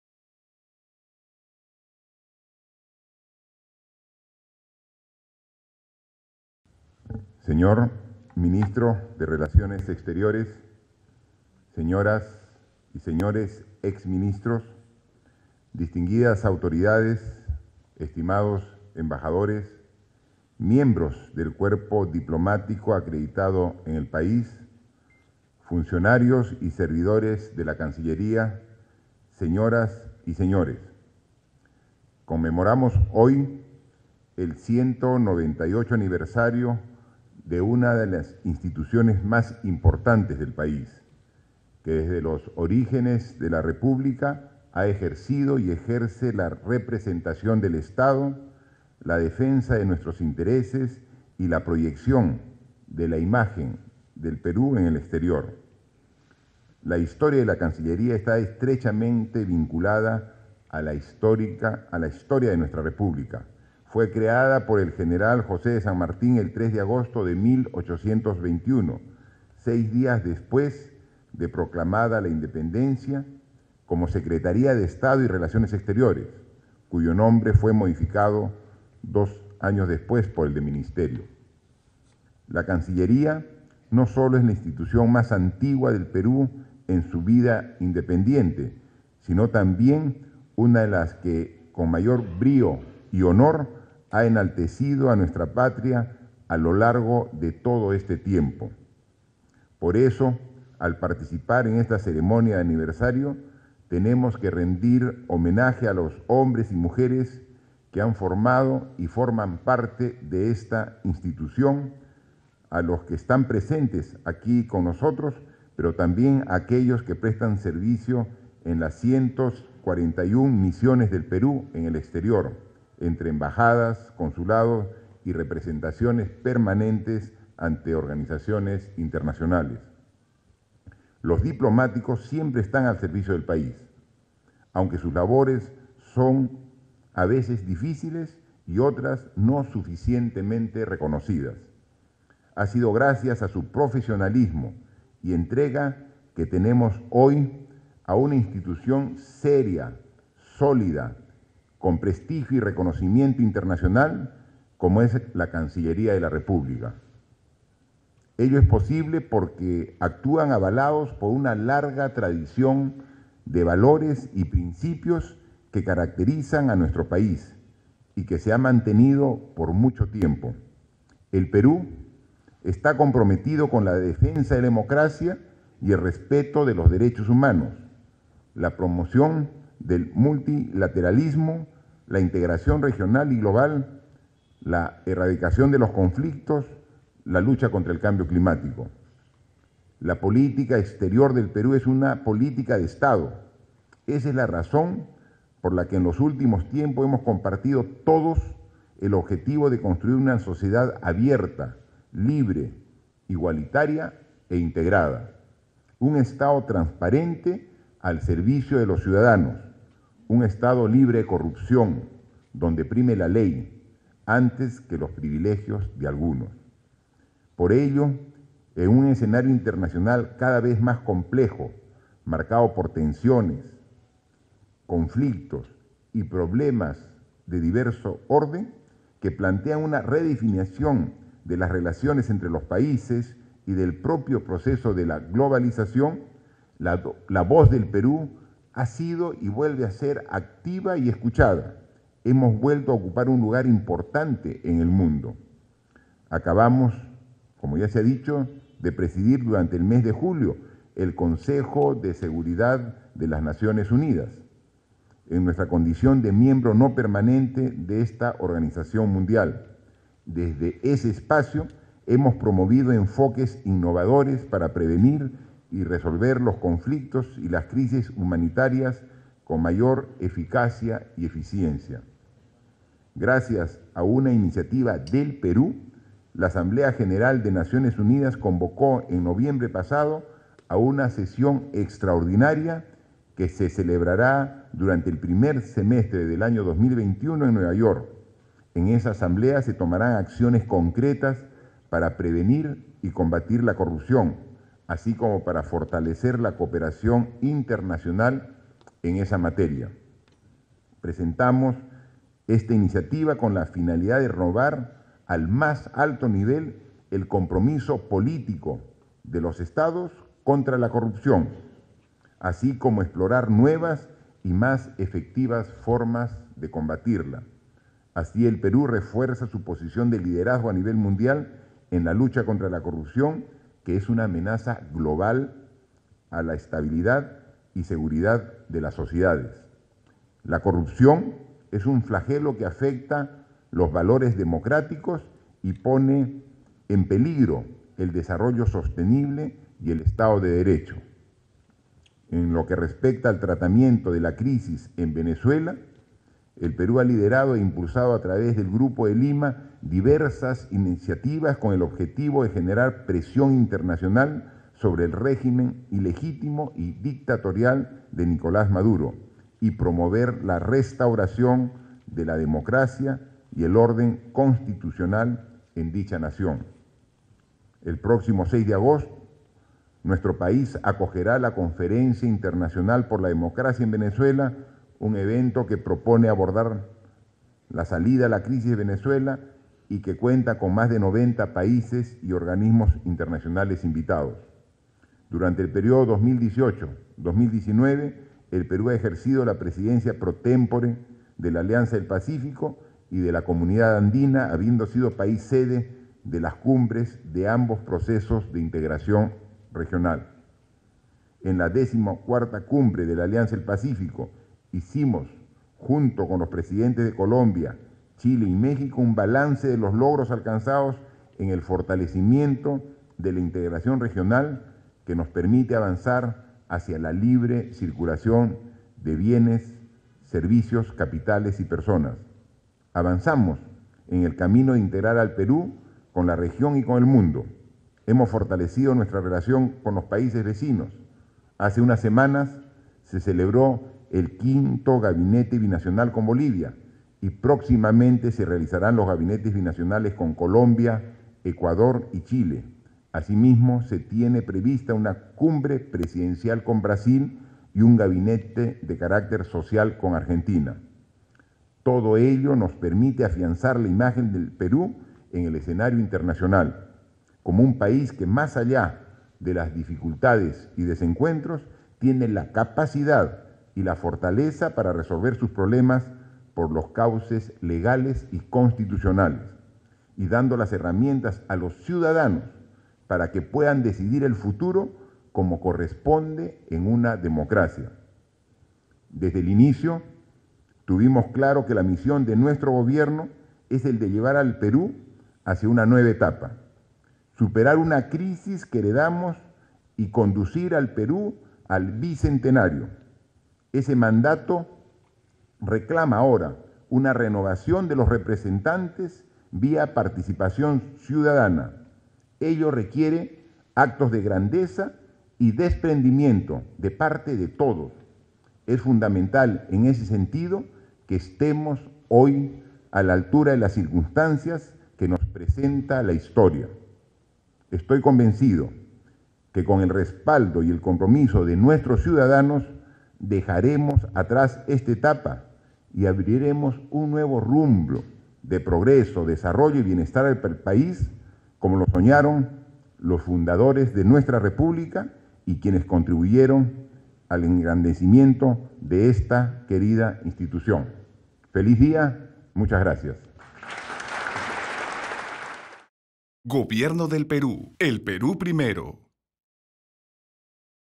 Ceremonia por el 198° Aniversario del Ministerio de Relaciones Exteriores
El Jefe de Estado asistió a la ceremonia desarrollada por el 198° Aniversario del Ministerio de Relaciones Exteriores, oportunidad en la cual reafirmó el compromiso del gobierno en la lucha contra la corrupción.

Al respecto, informó que gracias a una iniciativa del Perú, en la próxima Asamblea General de las Naciones Unidas que se celebrará durante el primer semestre de 2021, en Nueva York, se tomarán acciones concretas para prevenir y combatir la corrupción, así como para fortalecer la cooperación internacional en esa materia.